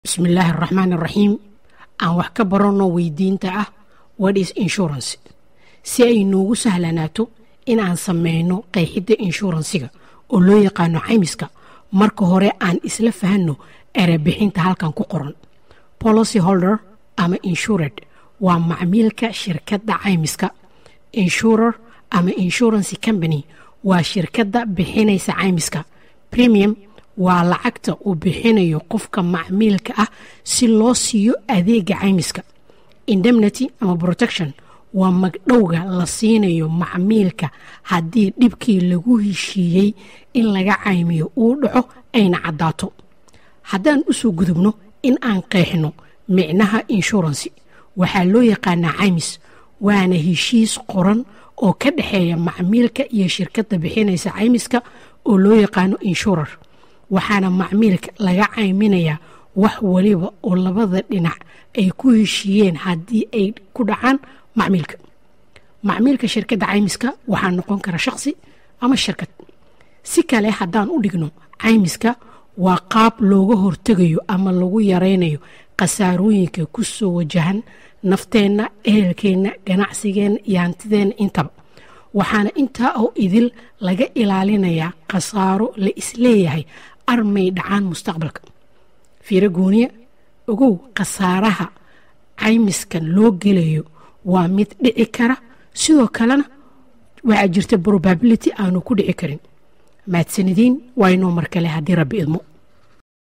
What is Insurance? Waa Maxay Ceymis? Kobciso_Academy Af - Somali
What is Insurance? Waa Maxay Ceymis?  Af - Somali

Insurance waa ceymis waana heshiis ka dhaxeeya macmiilka iyo shirkada bixisa ceymiska. Waana heshiis loogu talagalay in macmiilka looga ilaaliyo dhibaatooyin laga yaabo in ay dhacaan mustaqbalka. Ceymiska waa qeyb ka tirsan hey'adaha Maaliyada, si aad wax uga ogaatidna halkan ka dhageyso.